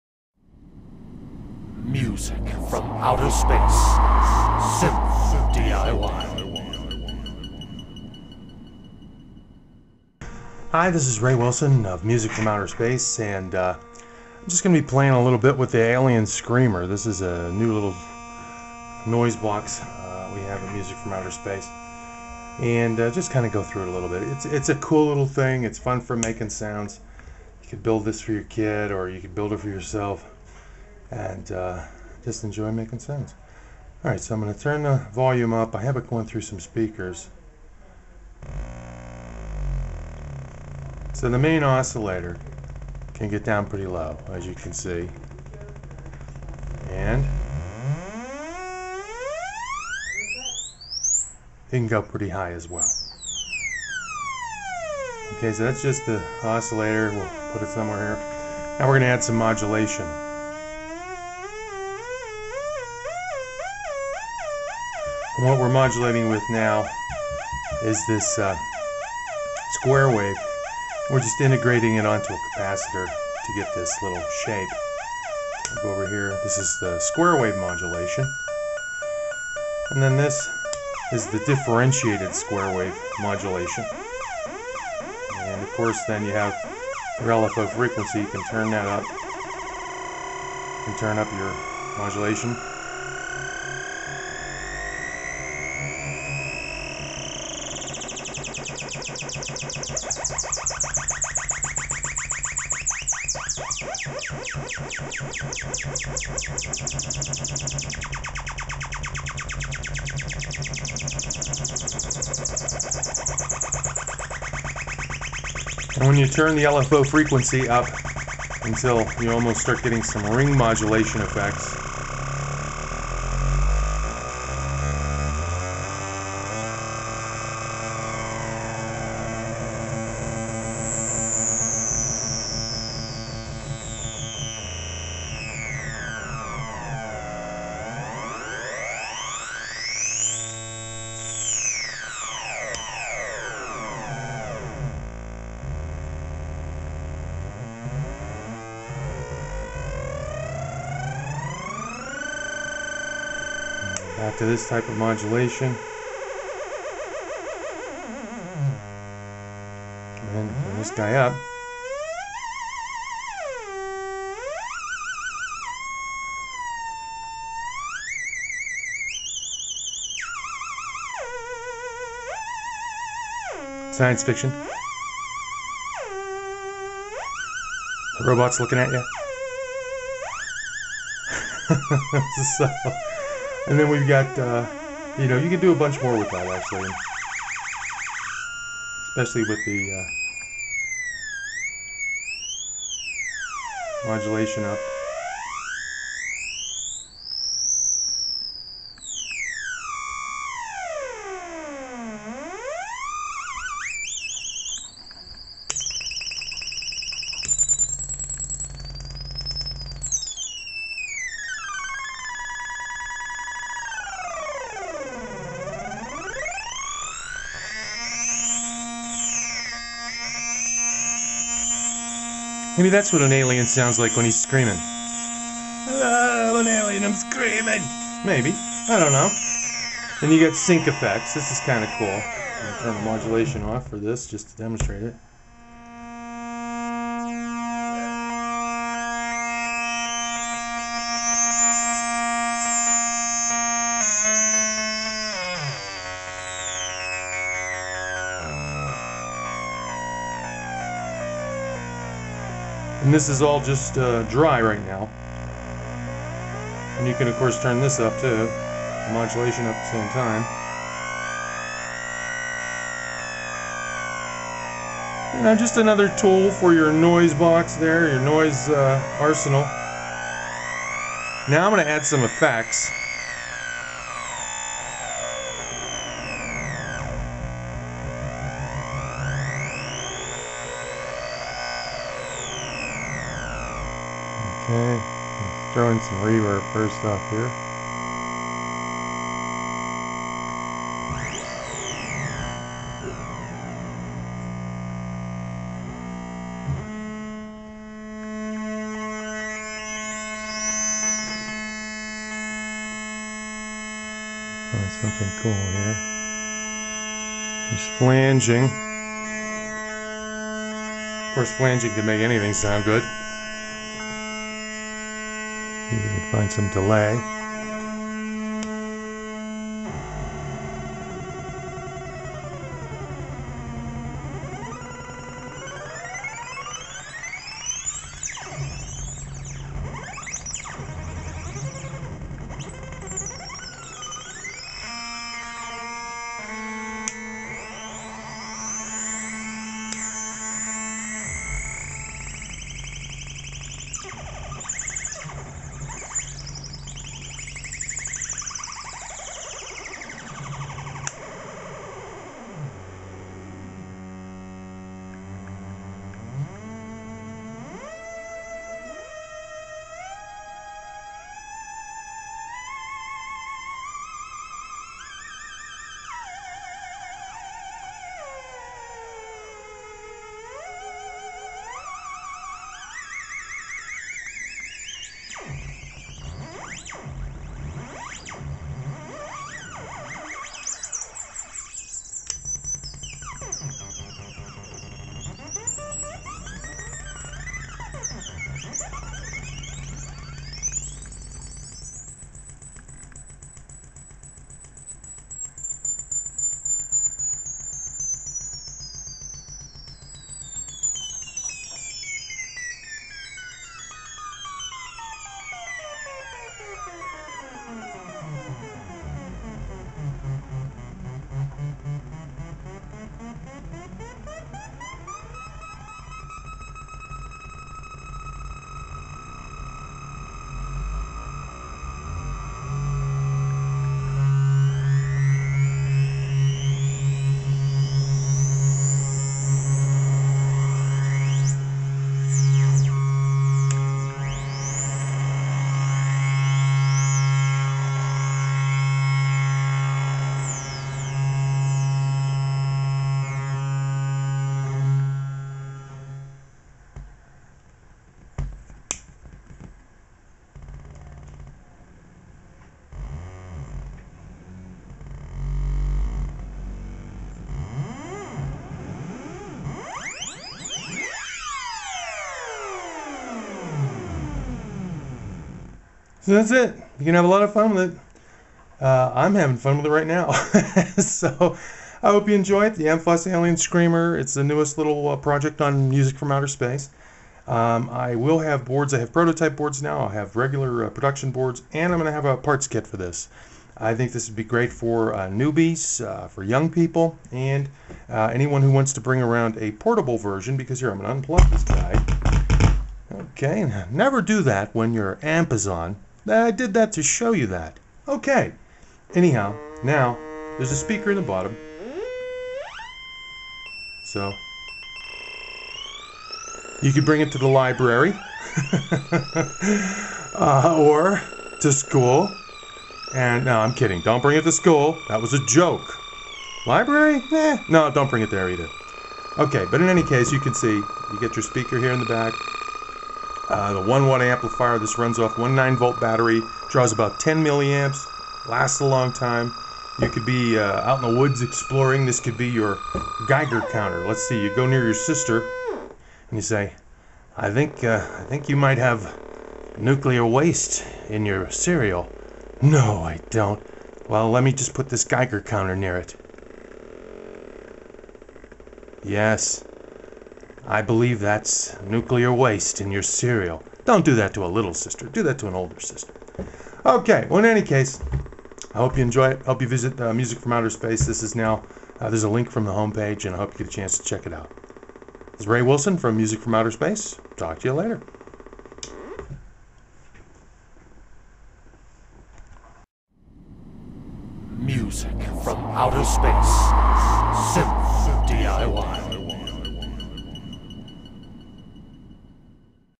MFOS Alien Screamer Noise Box Presentation
The MFOS Alien Screamer Noise Box is a great introduction to synth-diy electronics. This simple circuit can really make a lot of cool sounds and it has a speaker and amp built in. It runs off of one nine volt battery and draws very little current. PC boards are available and kits will be soon also.